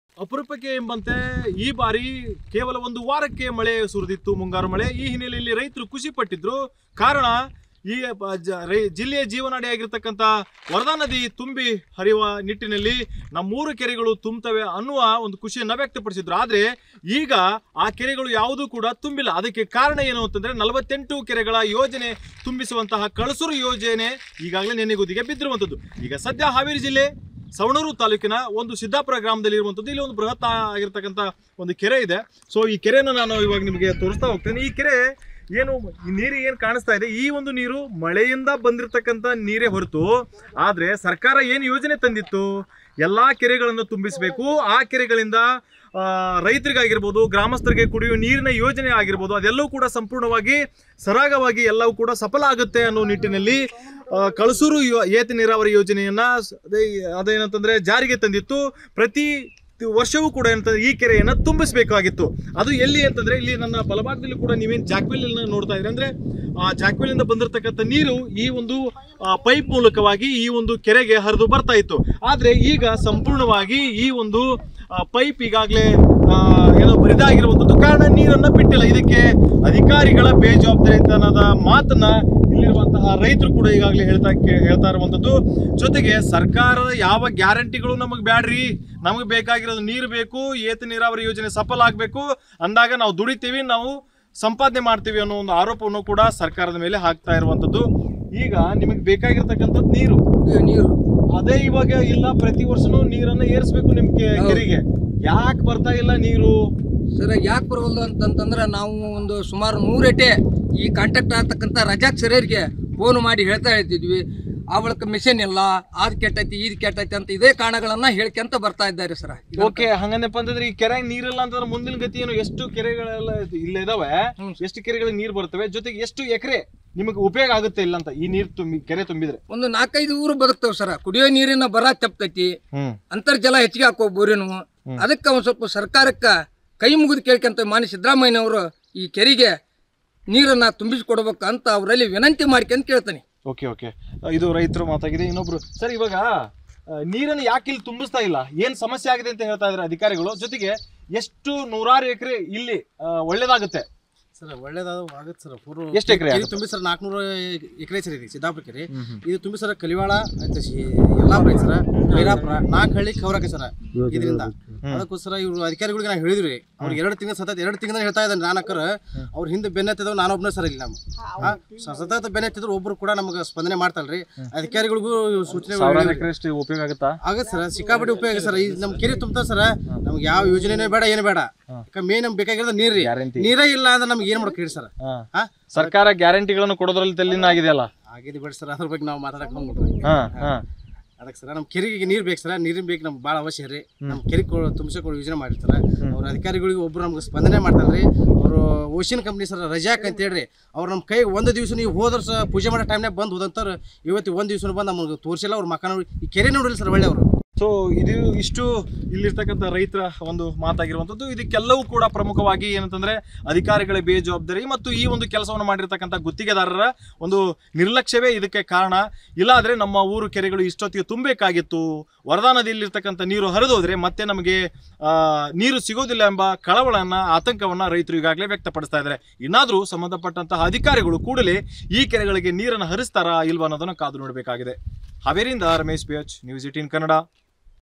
Savanur Lake Problem | ಬರಿದಾದ ಸಿದ್ದರಾಮಯ್ಯ ಕನಸಿನ ಕೂಸು ಸವಣೂರಿನ 48 ಕೆರೆ | Haveri News
Savanur Lake Problem | ಅಧಿಕಾರಿಗಳ ಬೇಜವಾಬ್ದಾರಿಗೆ ಸವಣೂರು ತಾಲೂಕಿನ 48 ಕೆರೆಗಳು ನೀರಿಲ್ಲದೆ ಒಣಗಿ ಹೋಗಿವೆ. ವರದಾ ನದಿಯಿಂದ ಸವಣೂರು ಏತ ನೀರಾವರಿ ಯೋಜನೆಯಲ್ಲಿ ನೀರು ತುಂಬಿಸಬೇಕು. ಆದ್ರೆ, ಪೈಪ್ ಹಾಳಾಗಿವೆ ,ಮಿಷನ್ ಕೆಟ್ಟು ನಿಂತಿವೆ ಎಂಬ ನಾನಾ ಕಾರಣಗಳನ್ನು ಹೇಳಿದ್ದಾರೆ. ಇದ್ರಿಂದ  ವರದಾ ನದಿಯ ನೀರು ಖಾಲಿ. | Haveri News

News18 Kannada Live | ನ್ಯೂಸ್ 18 ಕನ್ನಡ ಲೈವ್
Karnataka | ಕರ್ನಾಟಕ
Kannada | ಕನ್ನಡ